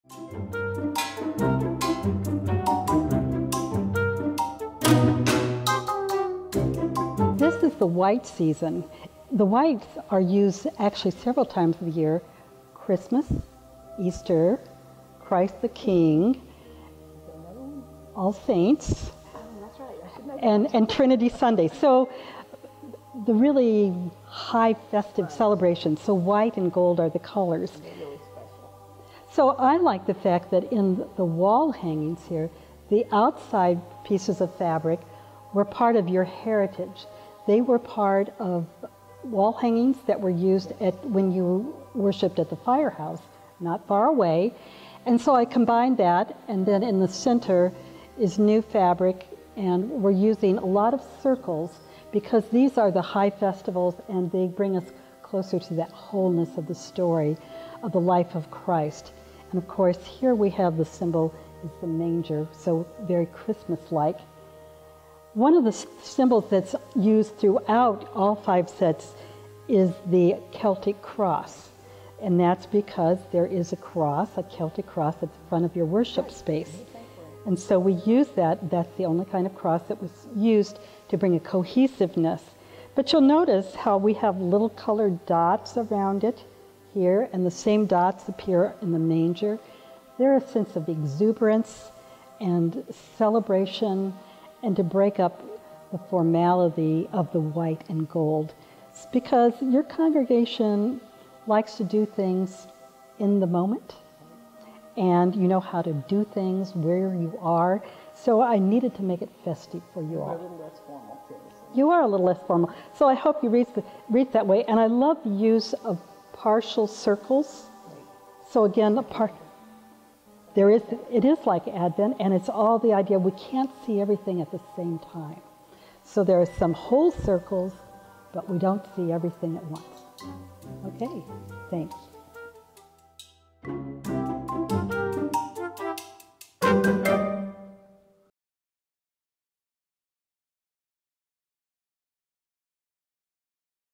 SOTH Paraments - White
Video 2 of 5 in the Paraments series. Textile artist, Norma Fredrickson and Lee Ann Walsh discuss the making, history, and meanings of the paraments made for Shepherd of the Hills Lutheran Church.